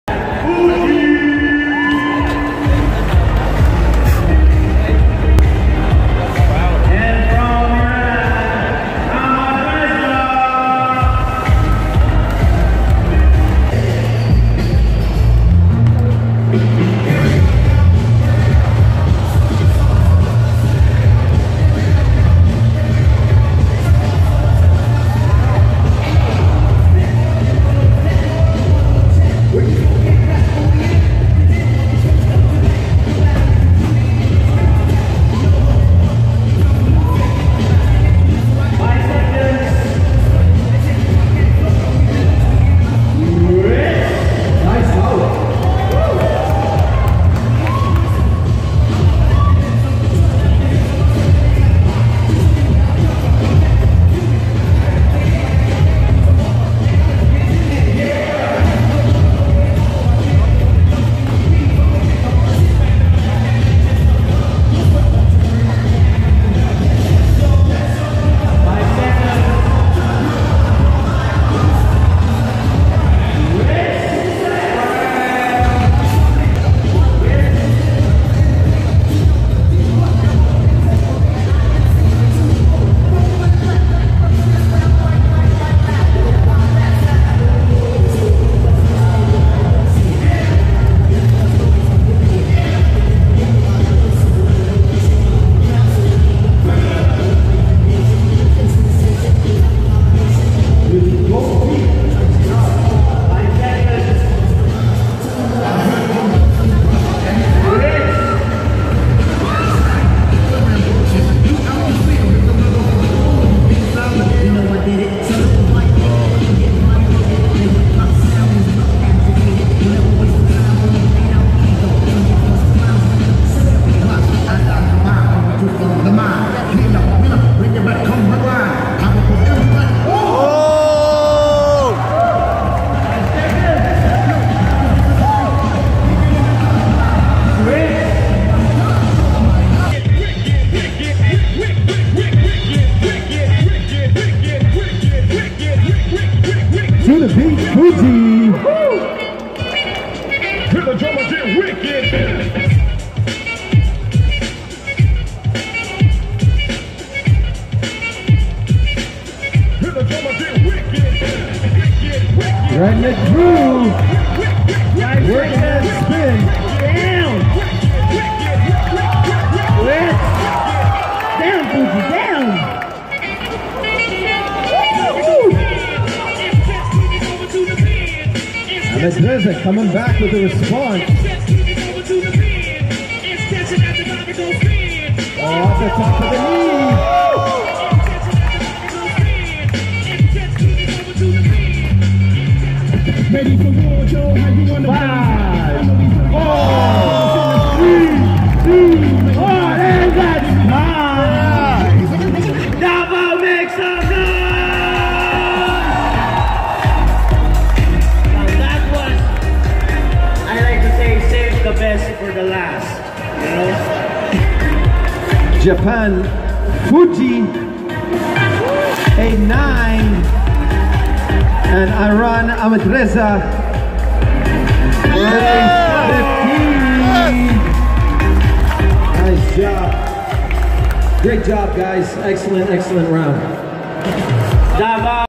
Ahmadreza (Iran) vs Fujji (Japon) / Asian Championship 2016 (Groupstage)
Asian Freestyle football Championship 2016
First place : Ahmadreza. (15)
Second place : Fujji (9)
Place : Philippines Davao city
Ahmadreza will accept all request performance in around the world.